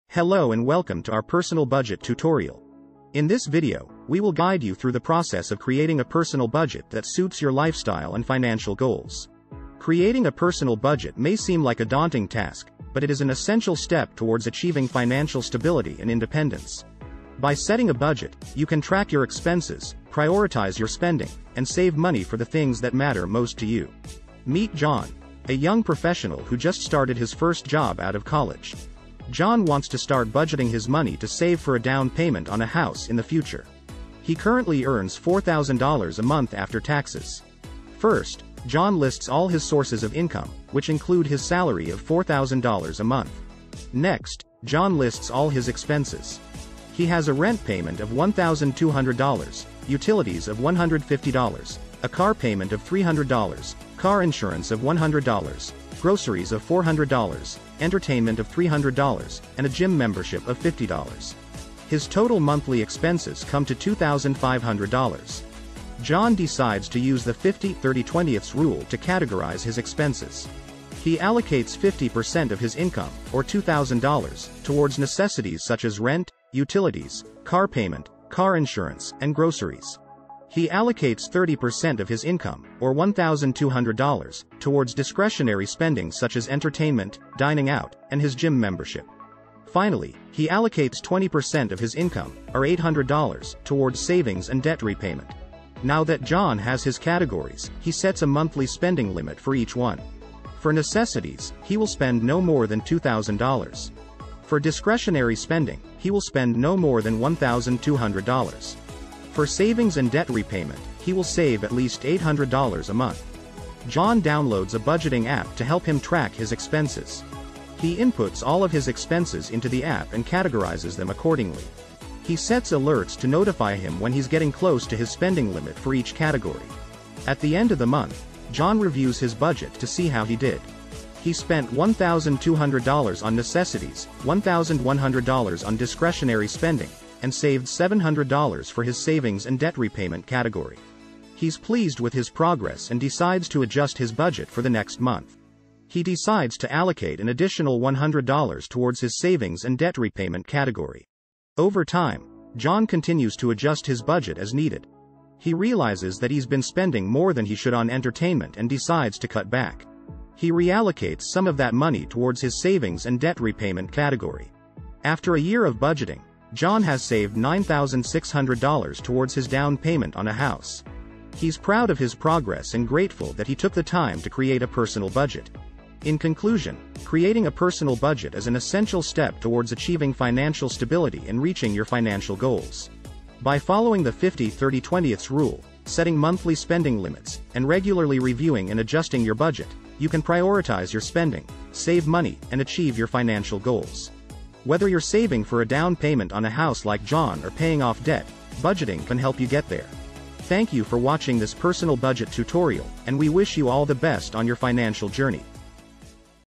Personal Budget Tutorial 2023 - Easy and Practical
Meet John, a young professional who just started his first job out of college. John wants to start budgeting his money to save for a down payment on a house in the future. He currently earns $4,000 a month after taxes.

First, John lists all his sources of income, which include his salary of $4,000 a month.

Next, John lists all his expenses. He has a rent payment of $1,200, utilities of $150, a car payment of $300, car insurance of $100, groceries of $400, entertainment of $300, and a gym membership of $50. His total monthly expenses come to $2,500.

John decides to use the 50/30/20 rule to categorize his expenses. He allocates 50% of his income, or $2,000, towards necessities such as rent, utilities, car payment, car insurance, and groceries. He allocates 30% of his income, or $1,200, towards discretionary spending such as entertainment, dining out, and his gym membership. Finally, he allocates 20% of his income, or $800, towards savings and debt repayment.

Now that John has his categories, he sets a monthly spending limit for each one. For necessities, he will spend no more than $2,000. For discretionary spending, he will spend no more than $1,200. For savings and debt repayment, he will save at least $800 a month.

John downloads a budgeting app to help him track his expenses. He inputs all of his expenses into the app and categorizes them accordingly. He sets alerts to notify him when he's getting close to his spending limit for each category.

At the end of the month, John reviews his budget to see how he did. He spent $1,200 on necessities, $1,100 on discretionary spending, and saved $700 for his savings and debt repayment category. He's pleased with his progress and decides to adjust his budget for the next month. He decides to allocate an additional $100 towards his savings and debt repayment category.

Over time, John continues to adjust his budget as needed. He realizes that he's been spending more than he should on entertainment and decides to cut back. He reallocates some of that money towards his savings and debt repayment category.

After a year of budgeting, John has saved $9,600 towards his down payment on a house. He's proud of his progress and grateful that he took the time to create a personal budget.

In conclusion, creating a personal budget is an essential step towards achieving financial stability and reaching your financial goals. By following the 50/30/20 rule, setting monthly spending limits, and regularly reviewing and adjusting your budget, you can prioritize your spending, save money, and achieve your financial goals. Whether you're saving for a down payment on a house like John or paying off debt, budgeting can help you get there.